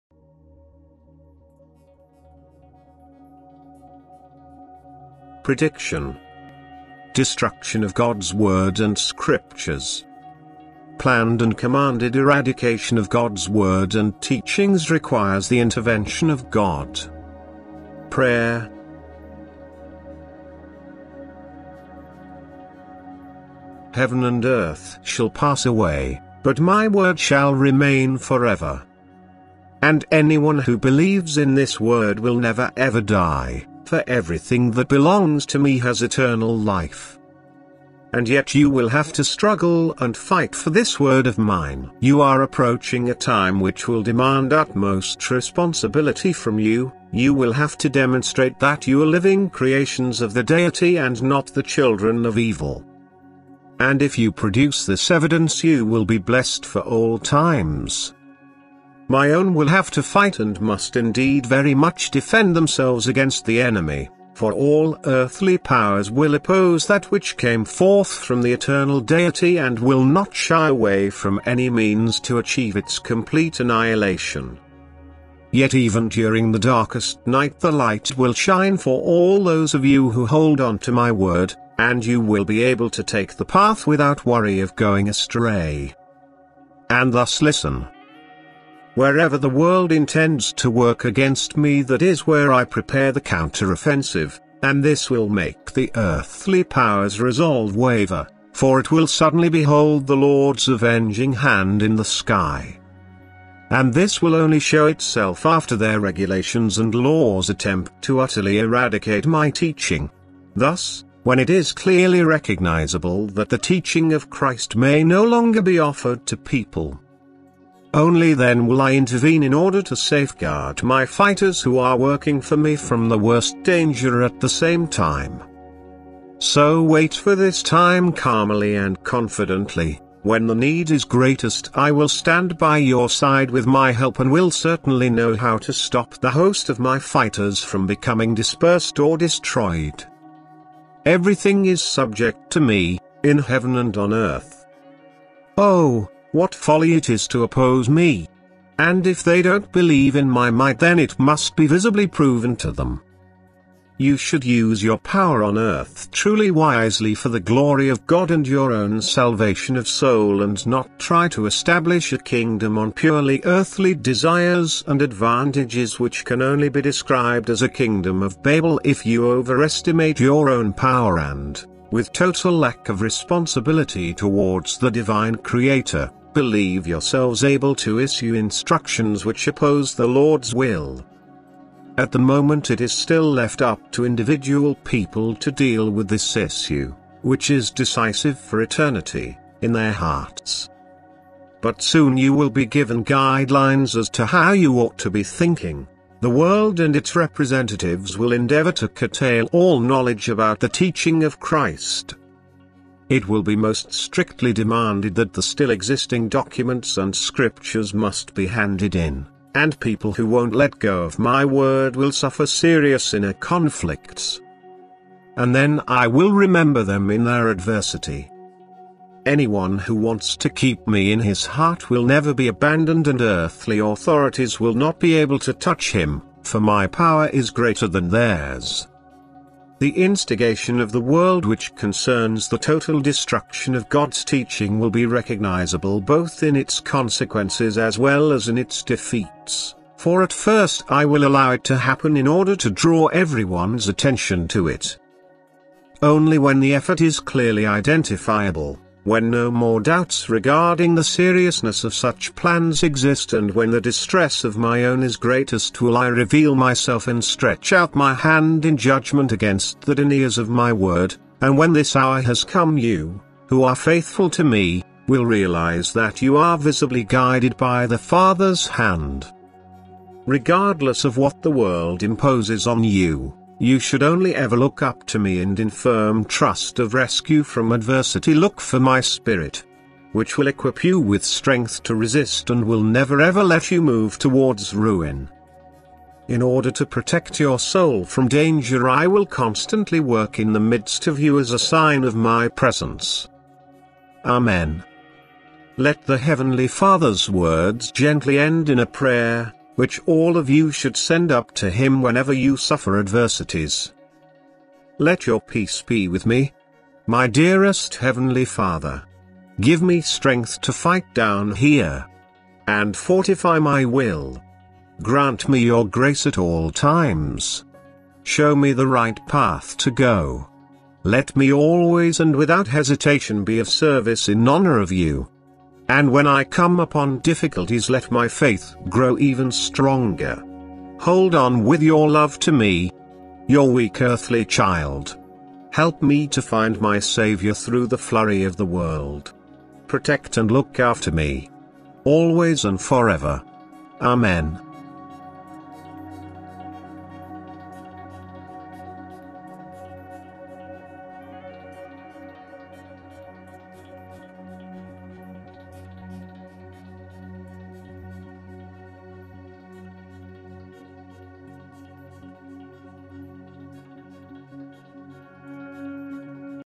PREDICTION ....DESTRUCTION of GOD´s WORD and SCRIPTURES .... PRAYER ....
END TIME PROPHECY
Revelation of Jesus Christ in our days
GOD corrected - FALSE DOCTRINES & ERRORS of the CHURCHES

Revelation 0641

"The Last seven Years of the earth"

A selection of God's revelations to Bertha Dudde, a seamstress from Schlesien, in this book, specifically on the events of the end times, received from 1937 to 1965 by the Inner Word according to

John 14.21 and Jesaja 42.9 and 48.6

These last seven years of the earth begin with the passing away of an earthly ruler and the subsequent outbreak of the Third World War, which God will then end with a worldwide unprecedented natural disaster.

God keeps his biblical promises as they are written: Isa.42.9

Behold, the former things are come to pass, and new things do I declare: before they spring forth I tell you of them.

Thou hast heard, see all this; and will not ye declare it ?

I have shewed thee new things from this time, even hidden things, and thou didst not know them.

Those who have not given their lives to Jesus Christ until today still
do so today because the end of this world on earth is near.